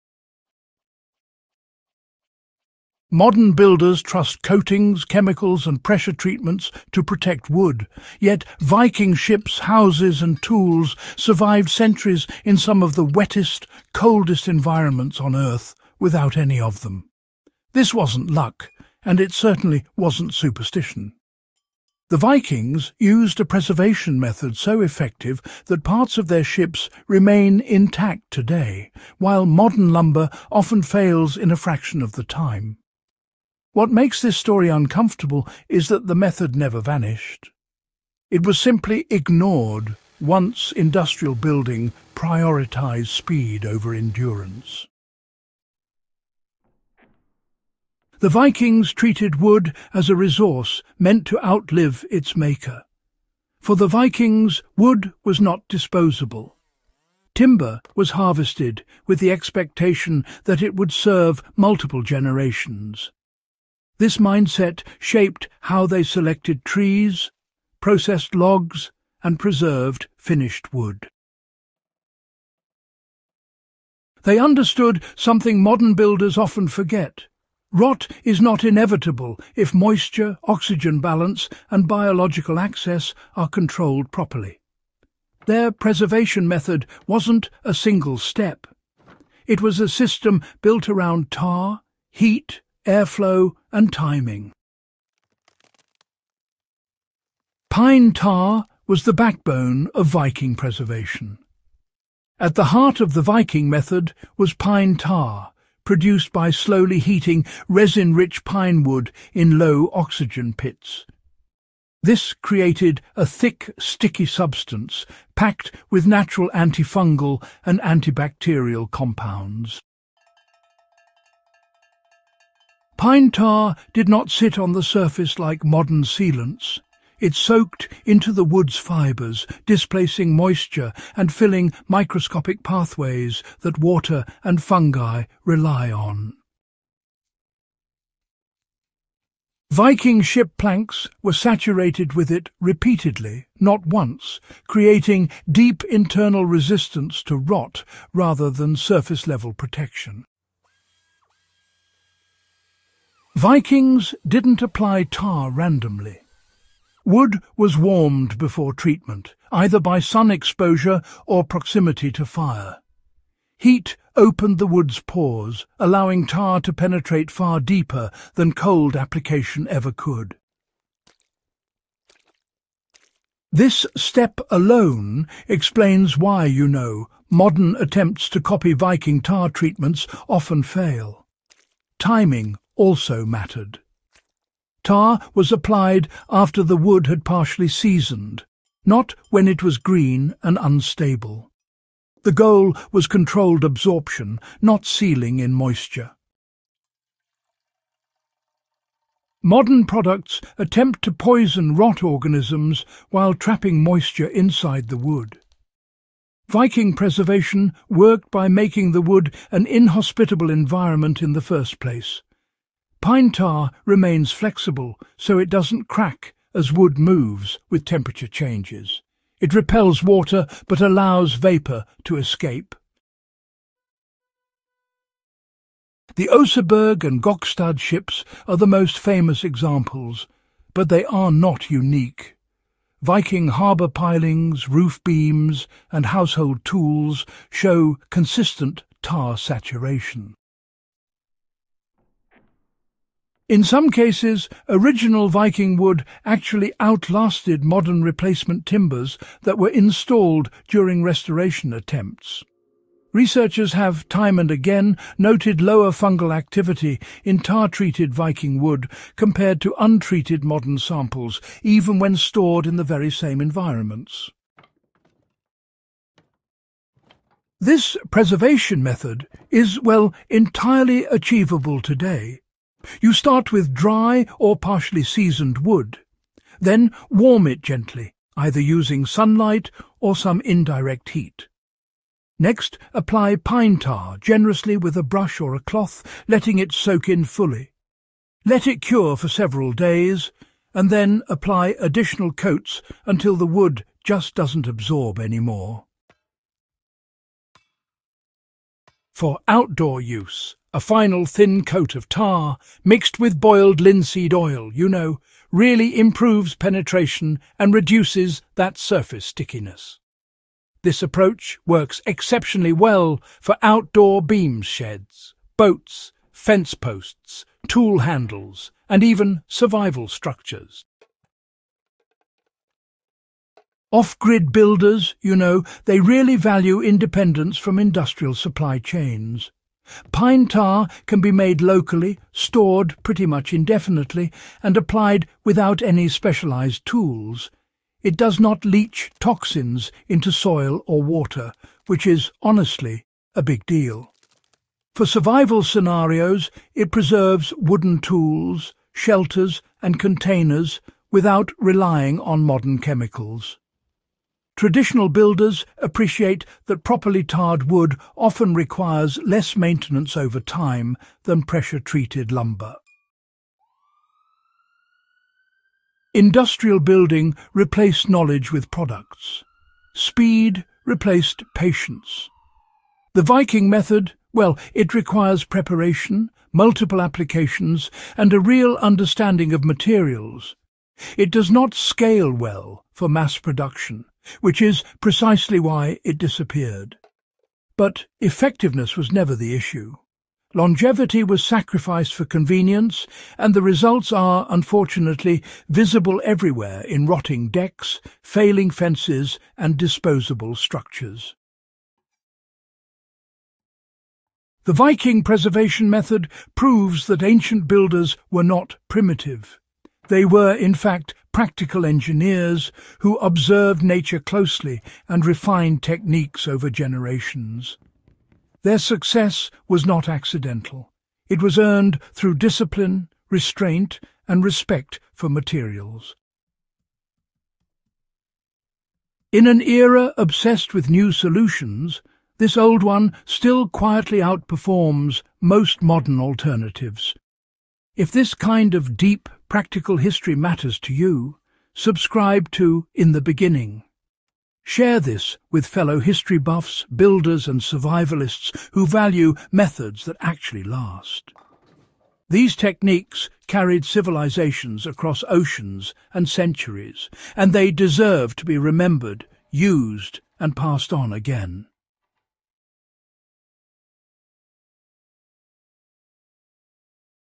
The Viking Preservation Method That Still Outperforms Modern Products
This Viking preservation method still outperforms modern wood products by sealing fibers instead of coating surfaces. It resists rot, moisture, fungi, and time better than chemical treatments.

Includes Viking woodworking, wood preservation, rot prevention, outdoor construction, and ancient material science.
This video uses AI narration and does not impersonate any individual.